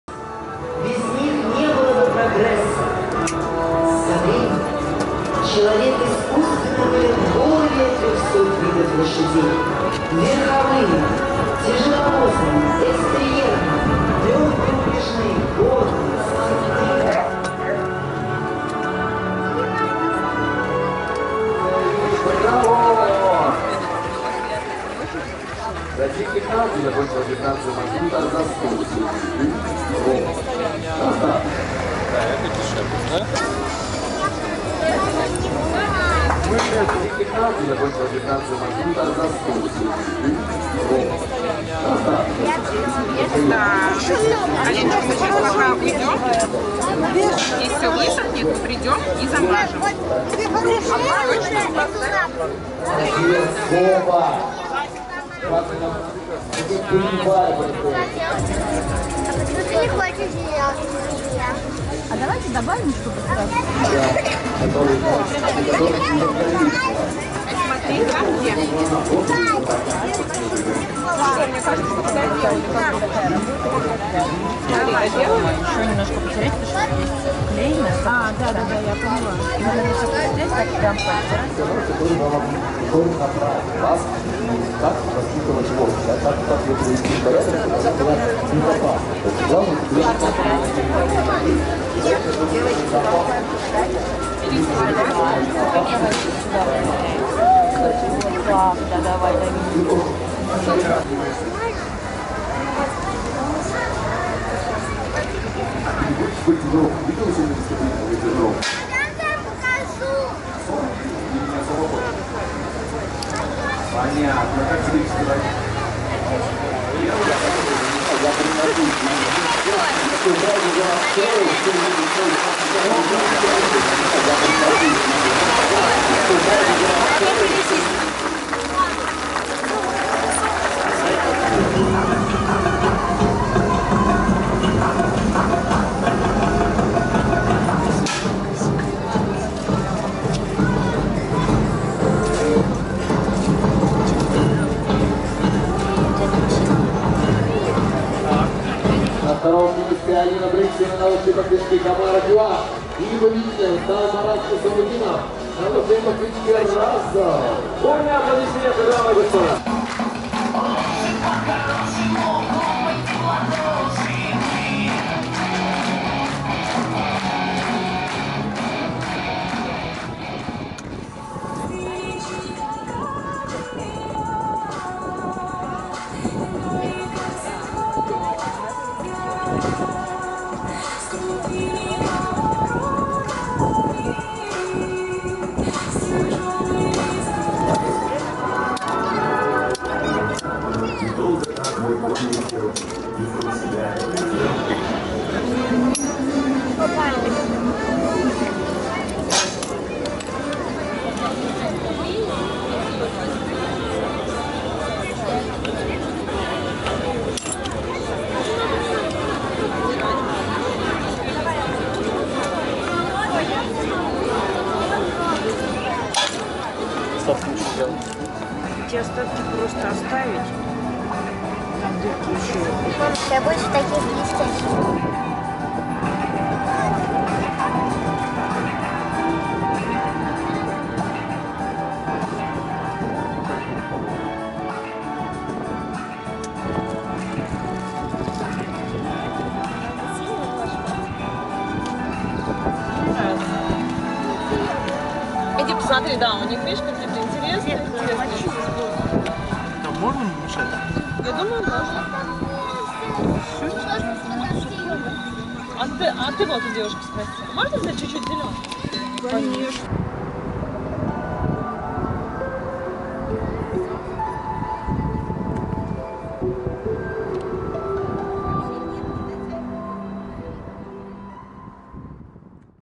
фестиваль Иваново Поле 2024 мастер класс по мозаике
19,20,21июля 2024
 мозаичная мастерская "В твоих руках"принимала участие в Шестом Конном Фестивале Иваново Поле.